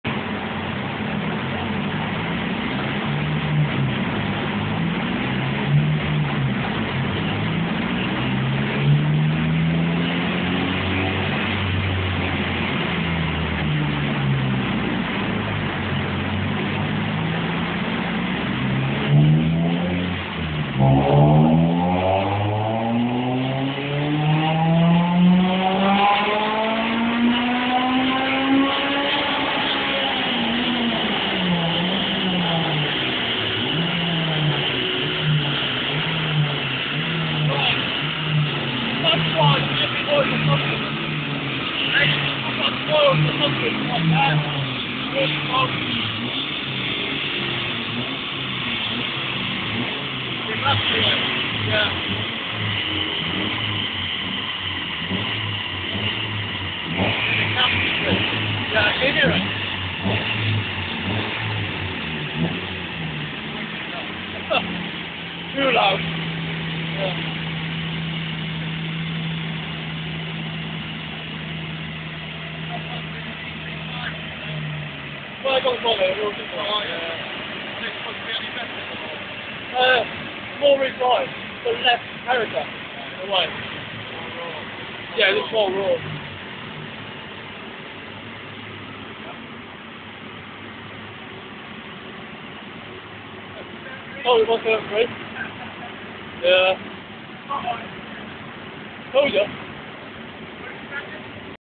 My First Dyno Run!!
I took my car to a RR days and did my first Dyno Run. Slightly dissapointed with the results but it was a fun and met a lot of nice people too!